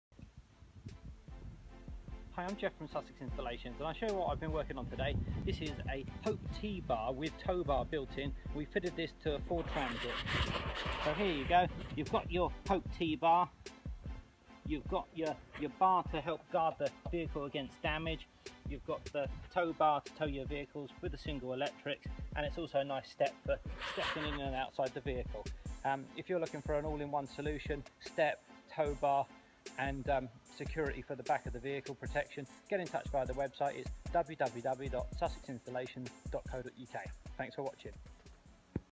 Hope Safe T-Bar - Van Step with Towbar installation to a 2008 Ford Transit in Eastbourne, Sussex
This video shows a 2008 Ford Transit that we fitted a Hope Safe-T Bar with built in towbar too.